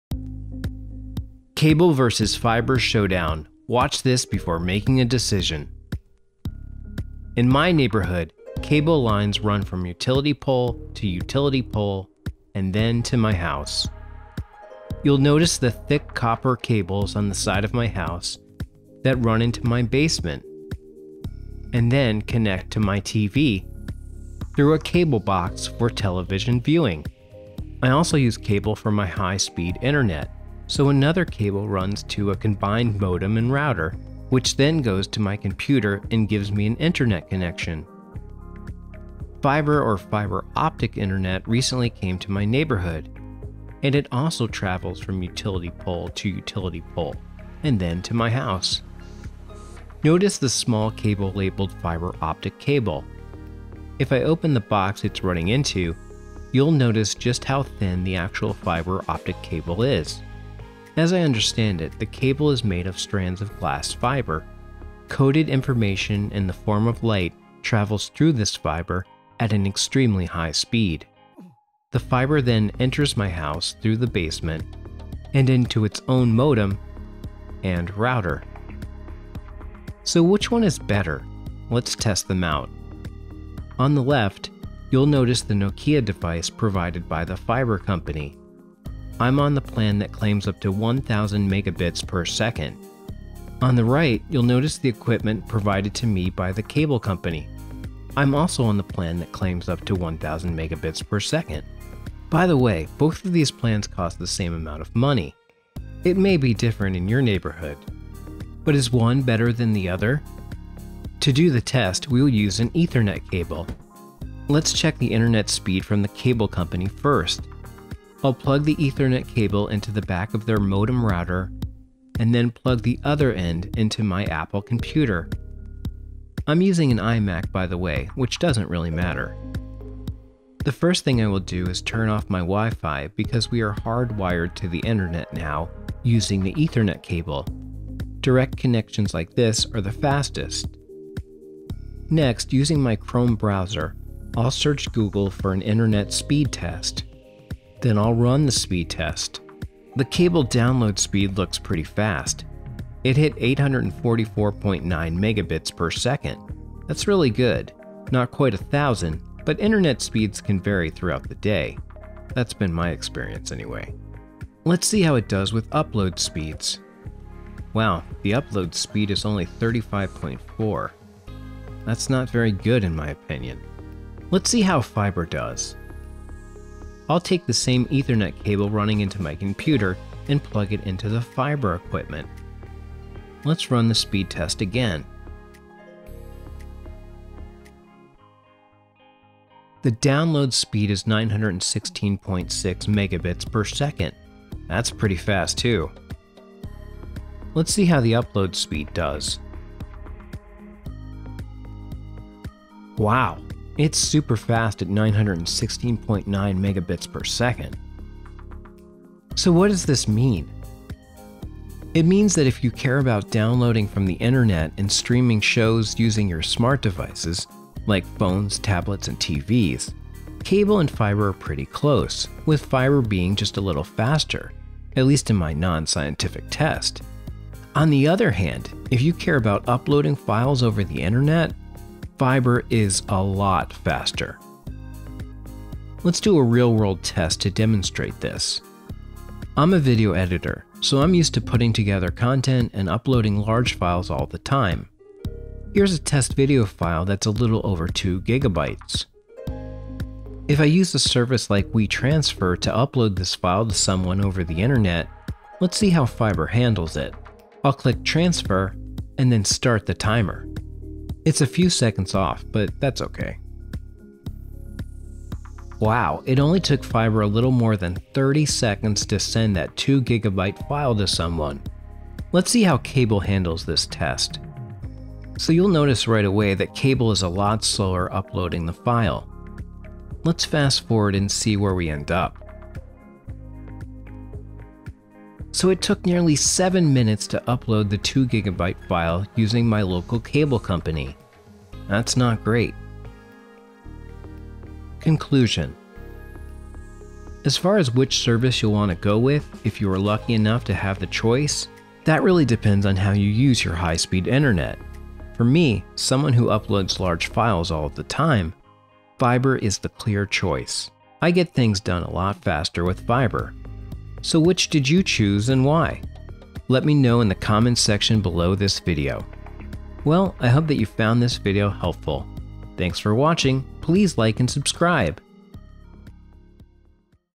Cable vs. Fiber Internet Showdown - Watch This Before You Make Your Choice!
Fiber or Fiber Optic Internet recently came to my neighborhood, so I went for it, but before getting rid of my Cable Internet, I decided to test them both out to find out which high speed internet option is the best!  Welcome to:  Cable vs. Fiber Internet Showdown!  You are going to want to watch this video before making your decision on which internet delivery system is better.